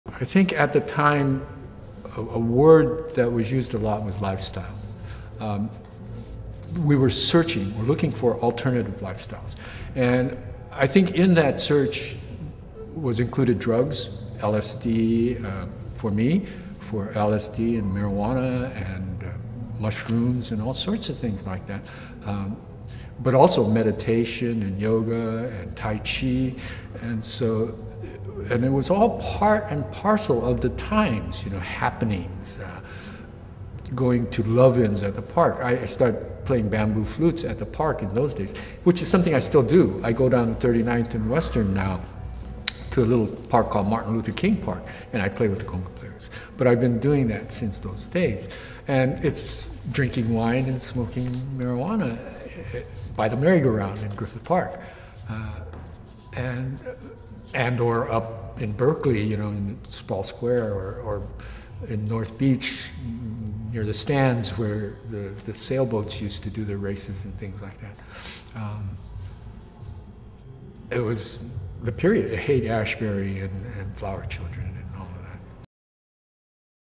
George Abe #5: Defining "Lifestyle"
Date: December 10, 2004
Location: California, US
Interviewer: Art Hansen, Sojin Kim
Contributed by: Watase Media Arts Center, Japanese American National Museum